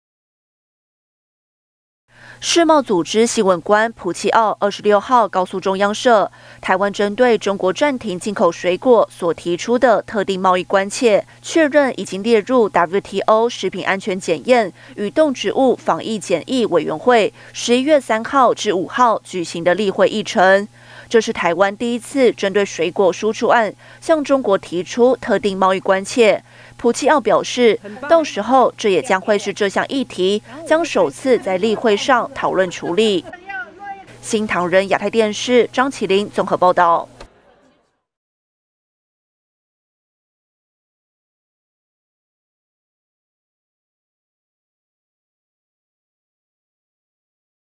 中共封禁台灣水果 WTO：已納入11月議程｜@ChinaBreakingNewsTV ｜20211028
世貿組織（WTO）新聞官普契奧（Fernando Puchol）26號告訴中央社，台灣針對中國暫停進口水果所提出的「特定貿易關切」（ specific trade concern，STC），確認已經列入WTO食品安全檢驗與動植物防疫檢疫（SPS）委員會11月3日至5日舉行的例會議程。

這是台灣第一次針對水果輸出案向中國提出「特定貿易關切」。普契奧表示，到時候這也將會是這項議題將首次在例會上討論處理。